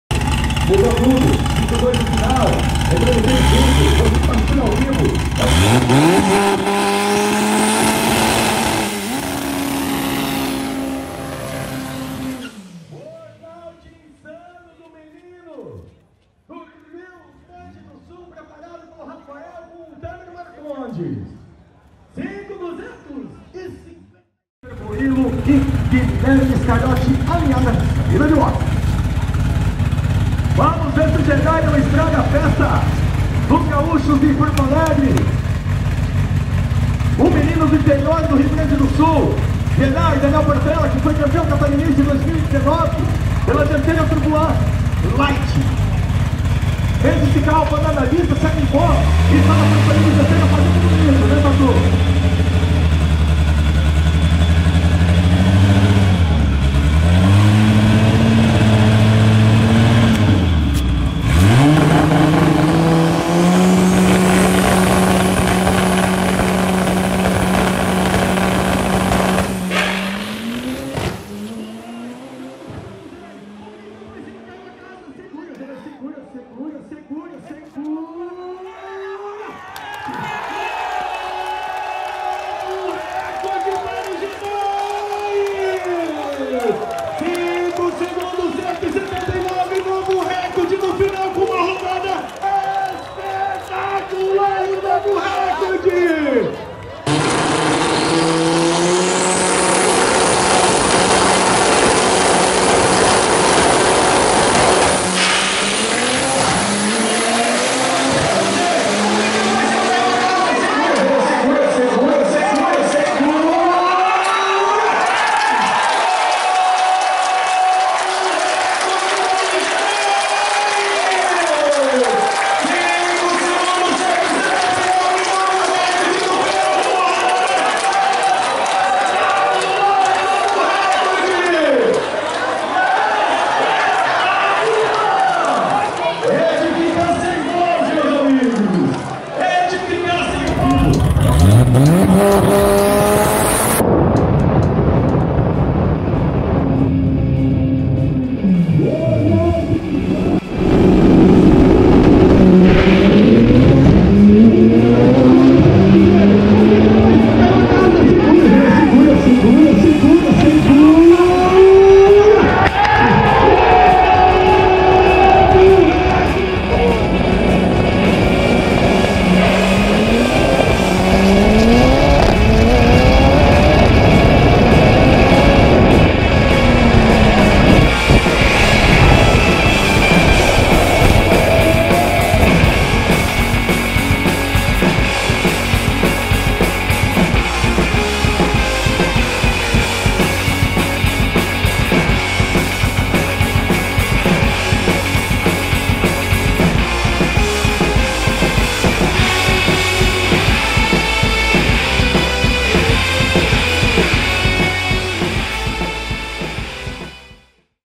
RECORDE DE DANIEL PORTELLA COM DIREITO A ZERINHO!
Daniel Portella bateu o recorde do Speedway Music Park na primeira etapa do Campeonato Catarinense de Arrancada na categoria FLD, e deu um enorme susto na galera no final da reta, mas tudo ocorreu bem sem nenhum dano material e físico ao piloto.

Créditos as imagens: Westcars e Racer Society

Thumb: @vinicausfotografia

Patrocínio:
INJEPRO